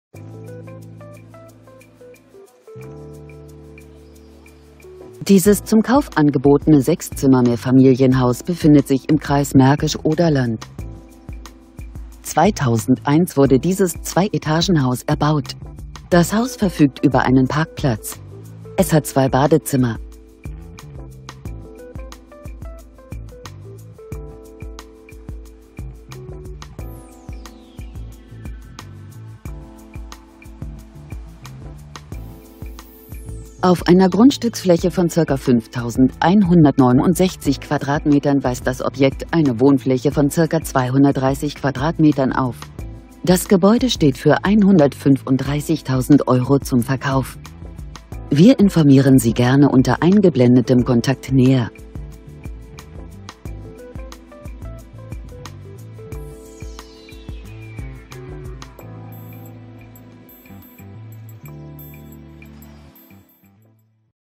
Gelbe Perle für zwei Familien
Mehrfamilienhaus
Preis: 135.000€
Zimmer: 6
Wohnfläche: ca. 230m²

 Fr. Brunhilde Lubrich

Grube
15320 Neutrebbin